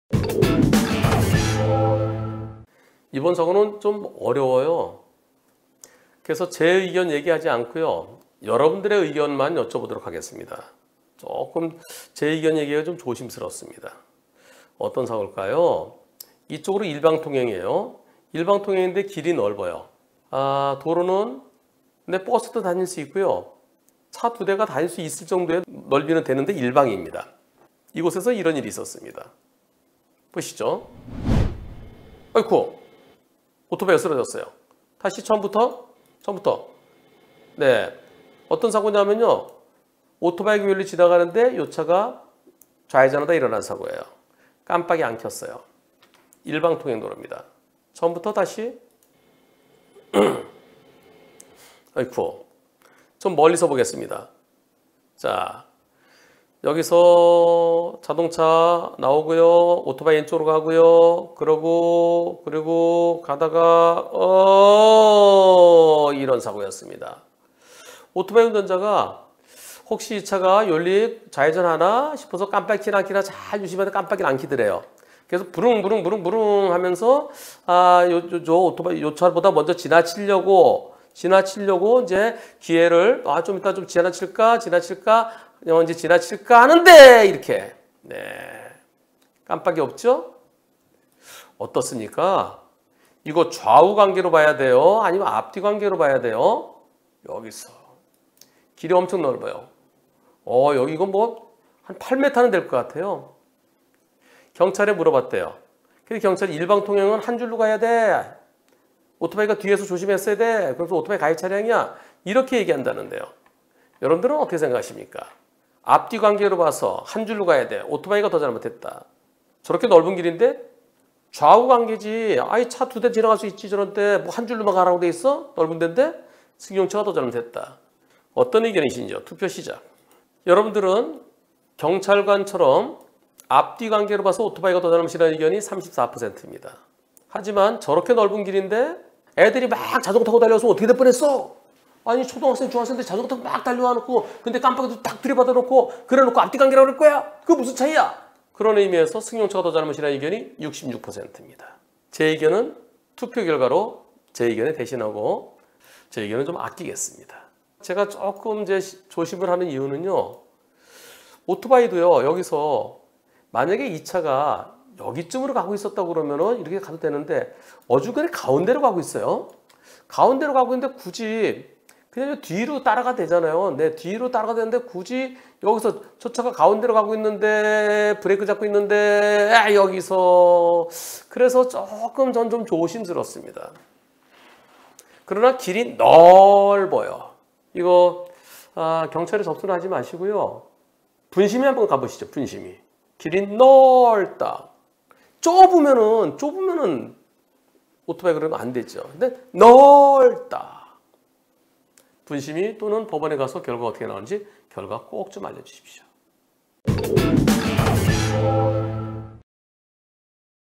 12795회. 오토바이가 자동차 옆 공간으로 앞지르려고 하는데 자동차가 갑자기 깜빡이 없이 좌회전하며 충돌. 누가 더 잘못인가요?
b17931
210911 (토) 오후 생방송
누가 더 잘못?

오토바이가 자동차를 앞지르려고 하는데 자동차가 갑자기 깜빡이 없이 좌회전하며 충돌

* 사고 내용 *

일방통행 길이긴 하지만 도로가 넓은 도로
상대 차량이 경차이다 보니 양쪽 공간엔 여유 공간이 좀 있는 상황이었고,
상대차량이 좌화전할 게 아니면 도로가 넓어지는 시점에 차량을 앞질러 가려고
상대방에게 저의 위치를 계속 보여주고 공회전까지 하며 소리를 일부러 내면서 브레이크 살짝살짝 잡으면서 진행, 자동차가 깜빡이 없이 꺾어 들어와 충돌.

아직 접수는 안됐지만 경찰서 가서 자문을 구해봤더니 오토바이가 가해자라고.

* 투표 *
1. 앞뒤 관계로 보아 오토바이가 더 잘못 (34%)
2. 좌우 관계로 보아 승용차가 더 잘못 (66%)

* 조심스럽다.
승용차가 가운데로 가고 있으면 오토바이가 뒤로 따라가면 될 텐데 굳이 앞질러 가려고 하다가..

경찰에 접수하지 말고 분심위 가보시길